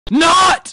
Ear Rape NOT Sound Effect
Here is a parody of the PBS Logos from the past years yelling, "NOT!!!" a lot. Feel free to use it for any video you want.